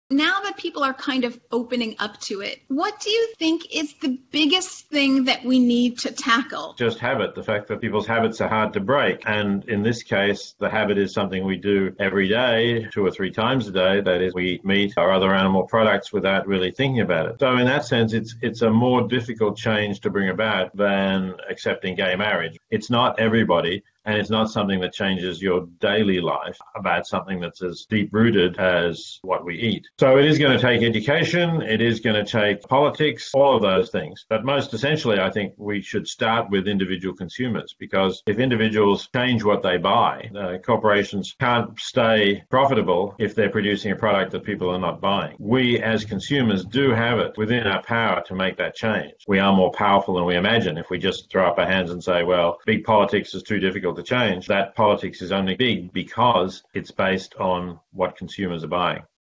Peter Singer on Taking on Big Politics
Famed author of Animal Liberation, Peter Singer on Taking on Big Politics on Awesme Vegans with Elysabeth Alfano.